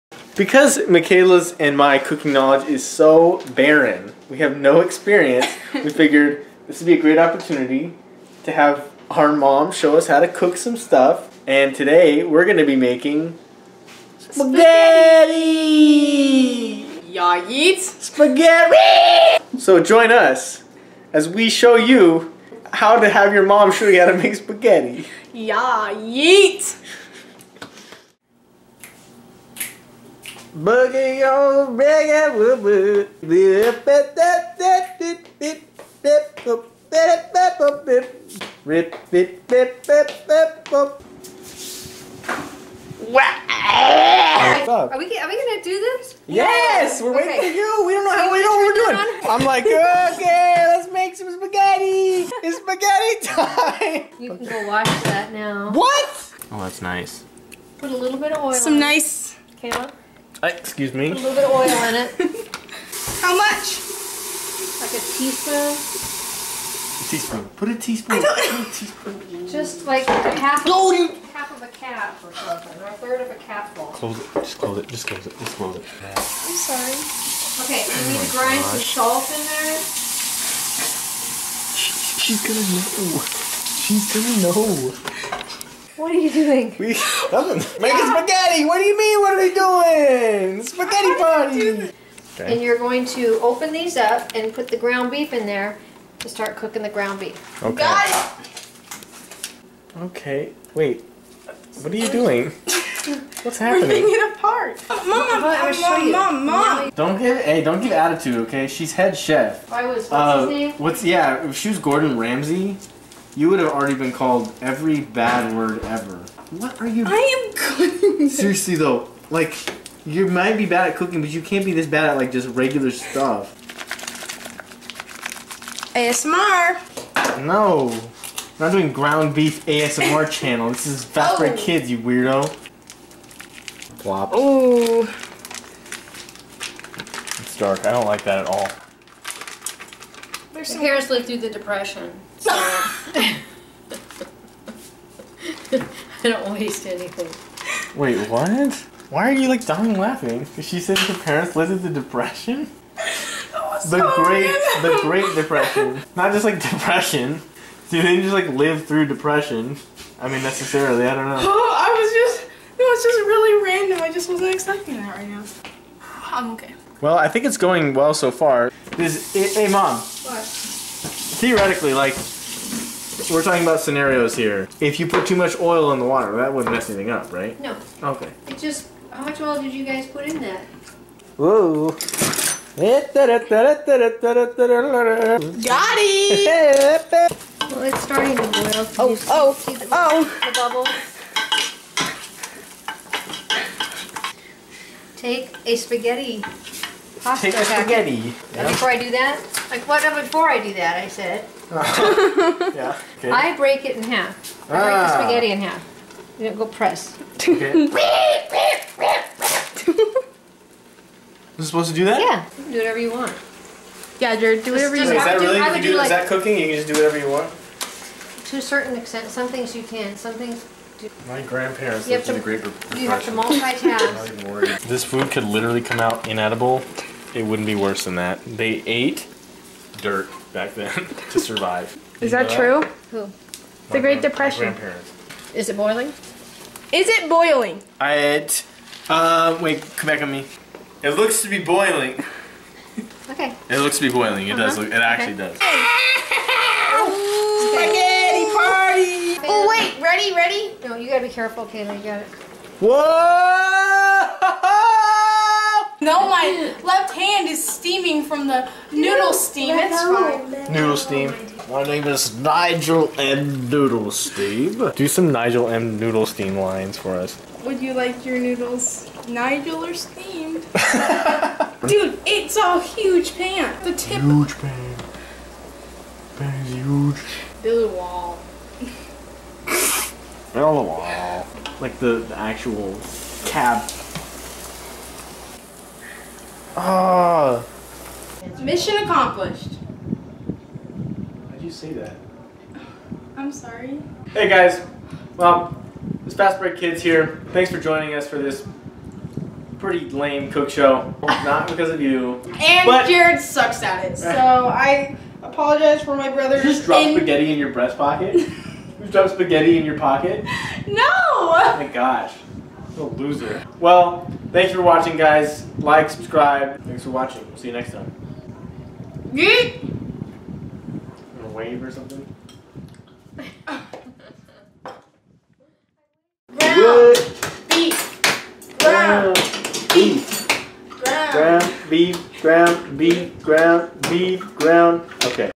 Spaghetti Time With Mom!!
Watch as our mom walks us through the simple task of preparing a basic spaghetti dish.

This is NOT ground beef ASMR.